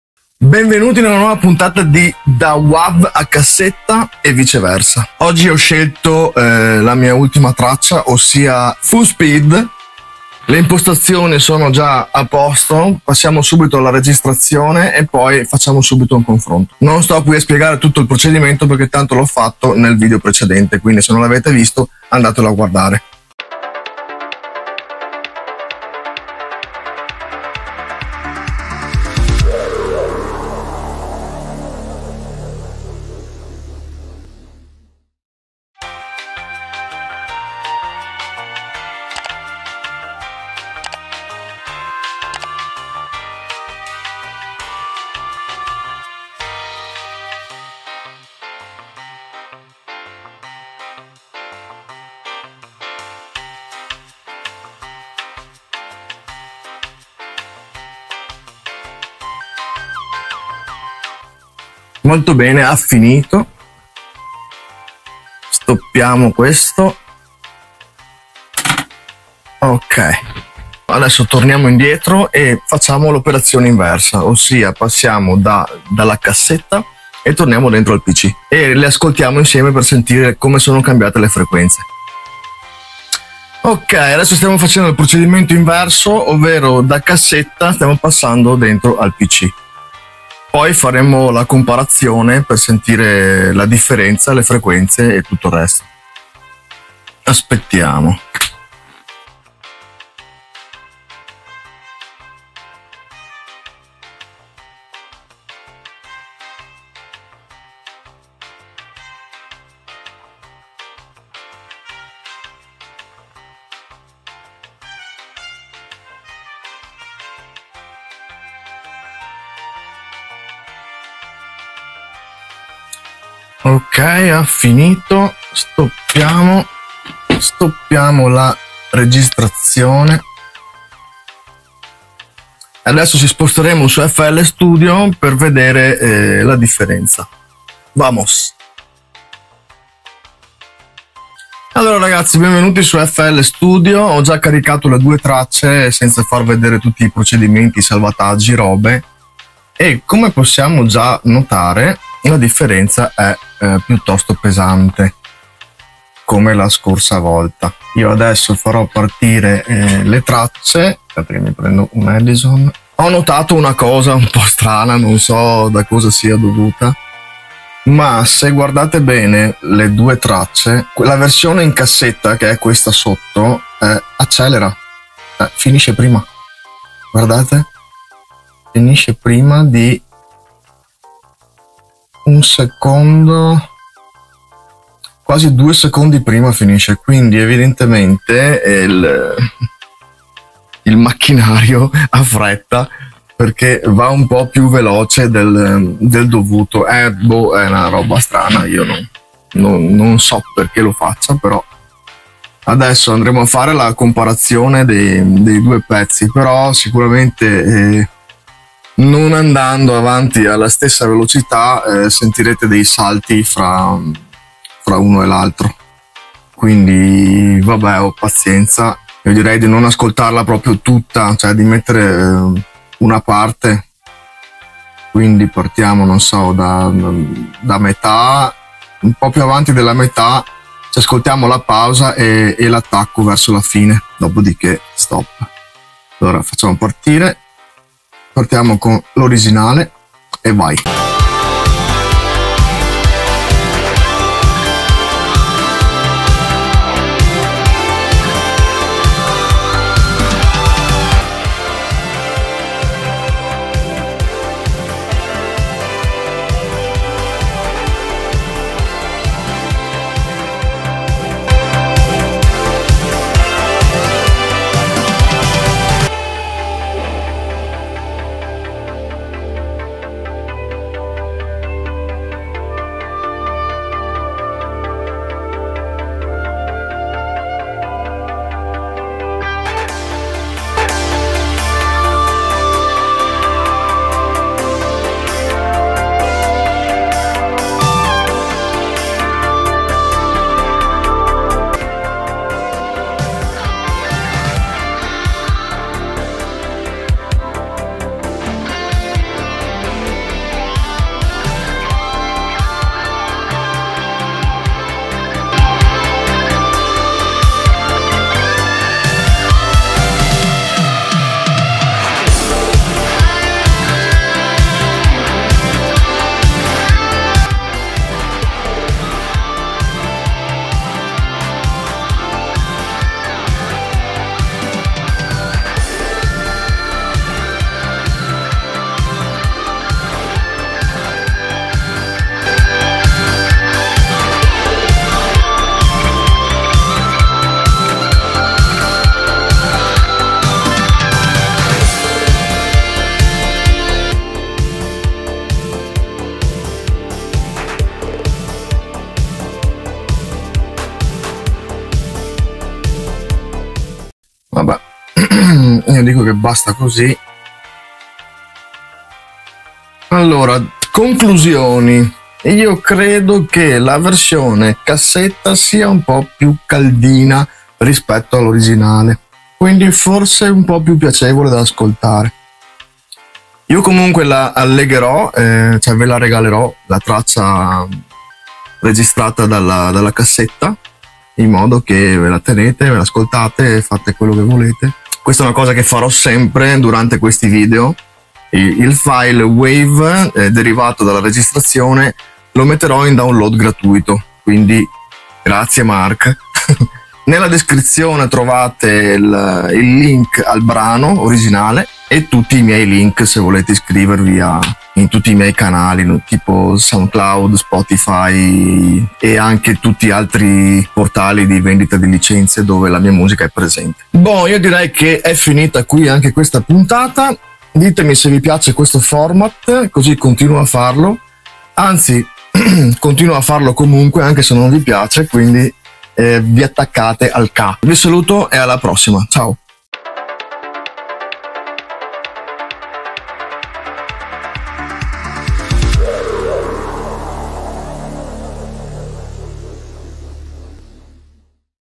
Porto una mia traccia da Wav a cassetta e viceversa... VOL.2
ATTIVATE I SOTTOTITOLI NELLA VOSTRA LINGUA

TURN ON SUBTITLES IN YOUR LANGUAGE

In questo video mostrerò il procedimento che eseguirò per passare una mia traccia da Wav a musicassetta in modo fisico, per poi tornare a registrare da musicassetta al PC, solo per acquistare il "dono" anni 80 e perdere alcune frequenze nel passaggio...

Linktree

Audiojungle

Jamendo